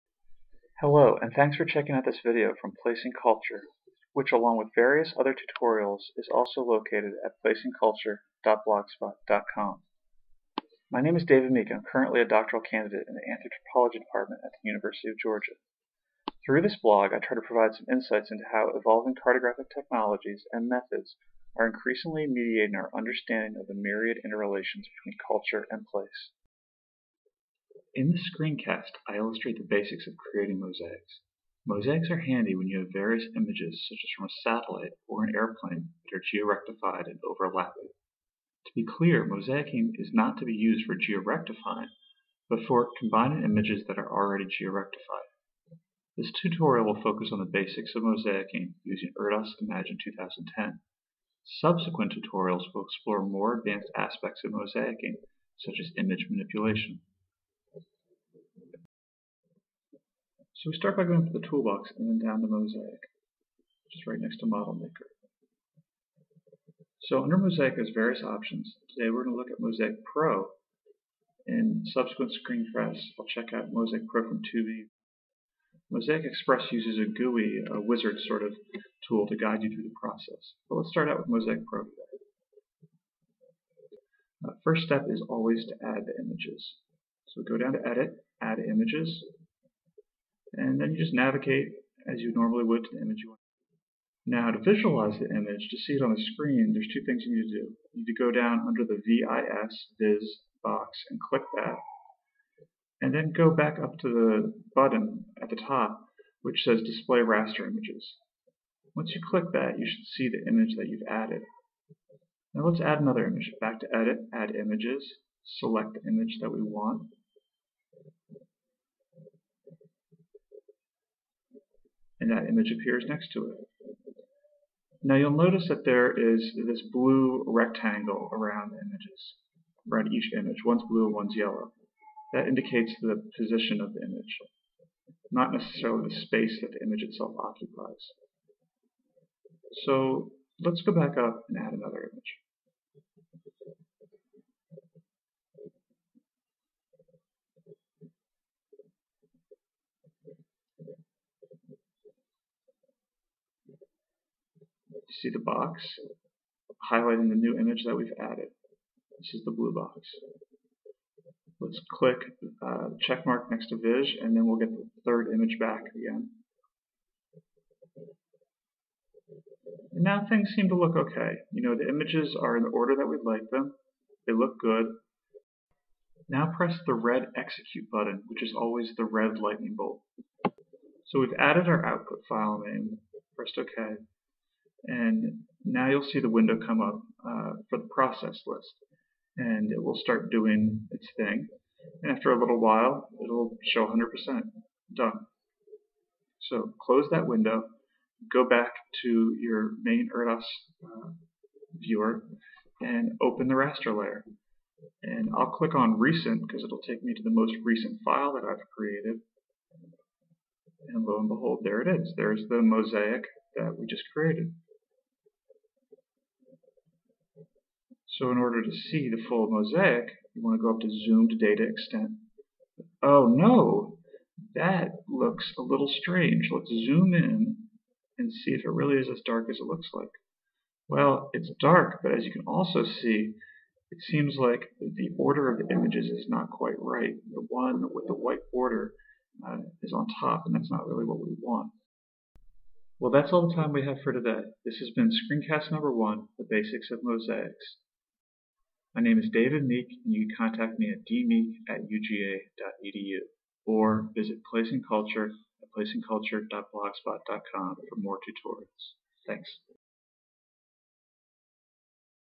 Basics of Mosaics with Erdas Imagine
The basics of creating a mosaic of georeferenced images using Erdas Imagine 2010.